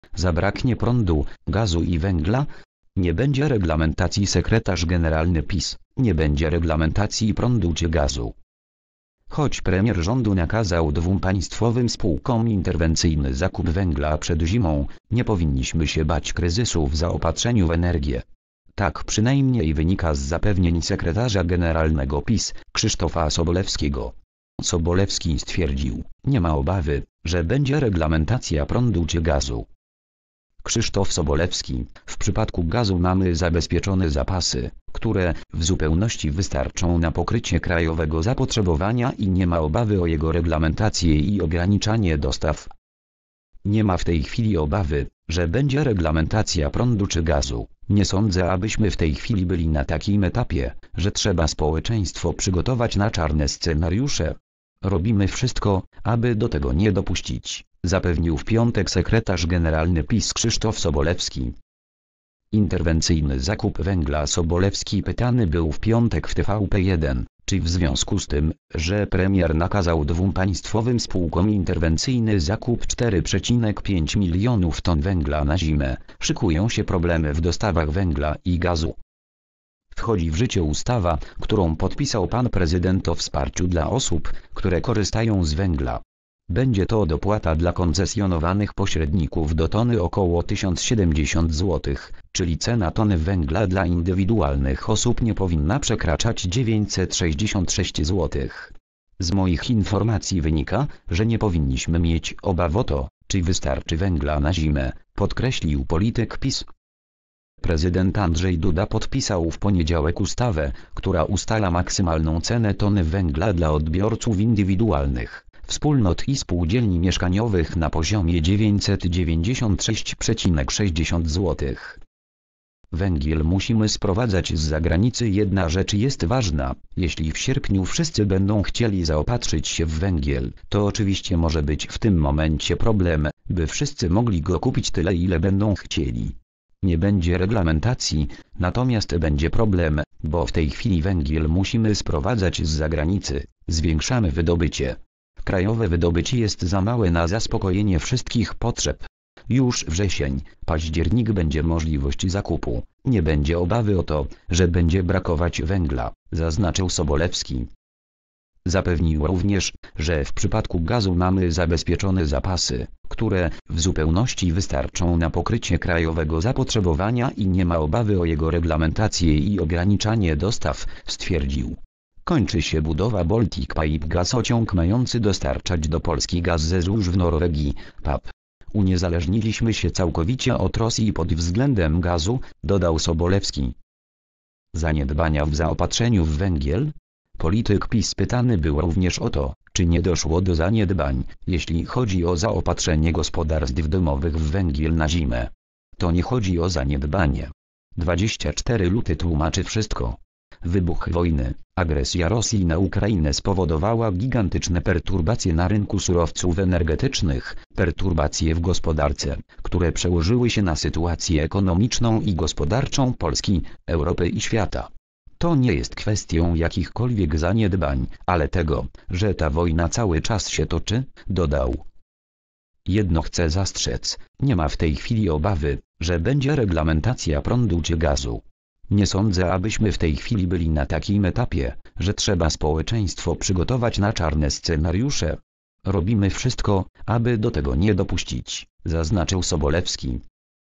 Zabraknie prądu, gazu i węgla? "Nie będzie reglamentacji"
Sekretarz generalny PiS: Nie będzie reglamentacji prądu czy gazu.

  (fot. Shutterstock)